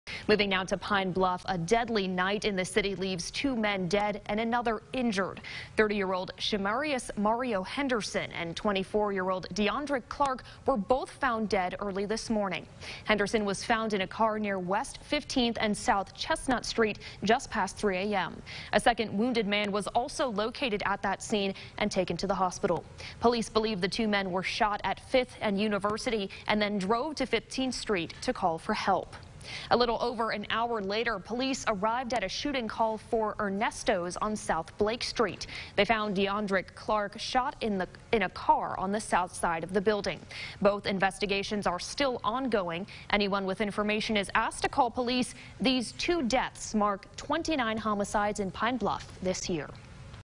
Two dead in Christmas morning shootings in Pine Bluff
Violence overnight has left two men dead and another person injured in Pine Bluff.